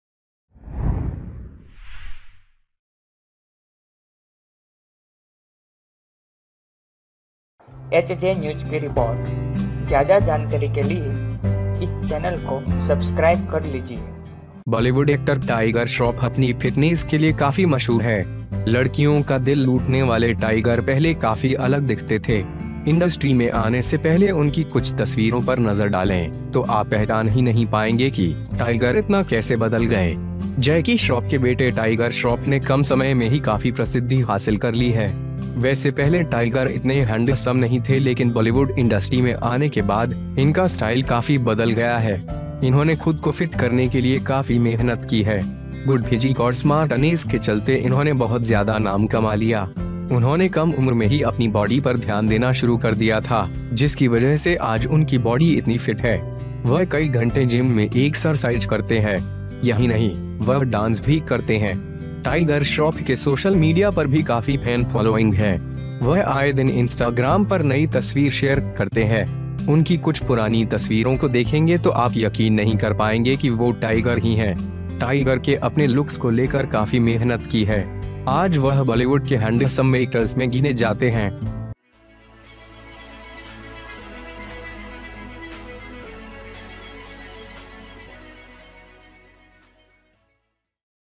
ये दुबला सा लड़का आज है बॉलीवुड का सबसे बड़ा सुपरस्टार, देखिये...
Bollywood से पहले ऐसे लगते थे टाइगर श्रॉफ, देखें...

Bollywood actor Tiger Shroff is famous for his fitness. Tiger who looted girls' hearts looked quite different before. Before coming to the industry, look at some of his photographs, you will not be able to recognize how much Tiger has changed so much.
Jackie Shroff's son Tiger Shroff has achieved great fame in a short span of time. Well earlier, Tiger was not so handsome but after coming to Bollywood industry, his style has changed quite a bit. He has worked hard to fit himself. Due to good physics and smartness, he earned a lot of name.
He started paying attention to his body at an early age, due to which his body is so fit today. He exercises several hours in the gym. Not only this, he also dances.
Tiger Shroff's social media is also following quite a fan. He shares a new picture on Instagram. If you look at some of his old photographs, you will not be able to believe that he is a Tiger.
Tiger has worked hard on his look. Today she is counted in Bollywood's Handsome Actors.

HJ NEWS !!